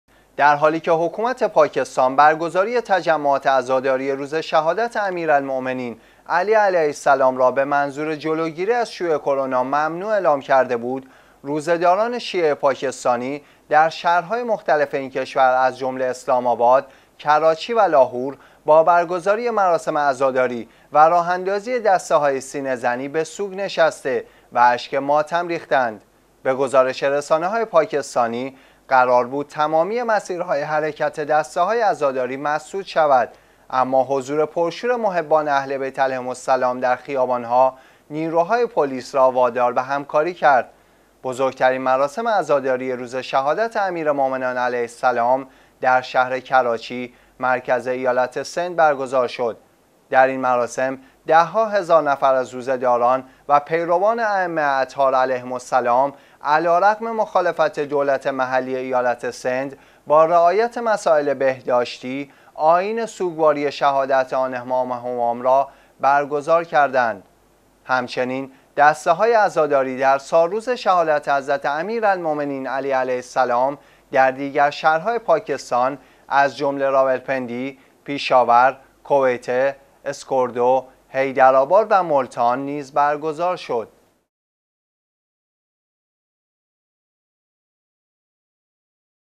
حضور عظیم جمعیت عزادارن در پاکستان/پلیس مجبور به همکاری شد
درحالیکه حکومت پاکستان برگزاری تجمعات عزاداری روز شهادت امیرالمومنین علی علیه السلام را به منظور جلوگیری از شیوع کرونا ممنوع اعلام کرده بود، روزه داران شیعه پاکستانی در شهر‌های مختلف این کشور از جمله اسلام آباد، کراچی و لاهور با برگزاری مراسم عزاداری و راه اندازی دست‌های سینه زنی به سوگ نشسته و اشک ماتم ریختند.
به گزارش رسانه‌های پاکستانی، قرار بود تمامی مسیر‌های حرکت دسته‌های عزاداری مسدود شود، اما حضور پرشور محبان اهل بیت علیهم السلام در خیابان ها، نیرو‌های پلیس را وادار به همکاری کرد.
بزرگترین مراسم عزاداری روز شهادت امیرمومنان علیه السلام در شهر کراچی مرکز ایالت سند برگزار شد.
که این مراسم ده‌ها هزار نفر از روزه داران و پیروان ائمه اطهار علیهم السلام علیرغم مخالفت دولت محلی ایالت سند با رعایت مسائل بهداشتی، آیین سوگواری شهادت آن امام همام را برگزار کردند.
همچنین دسته های عزاداری در سالروز شهادت حضرت امیرالمومنین علی علیه السلام در دیگر شهر‌های پاکستان از جمله راولپندی، پیشاور، کویته، اسکردو، حیدرآباد و مولتان نیز برگزار شد.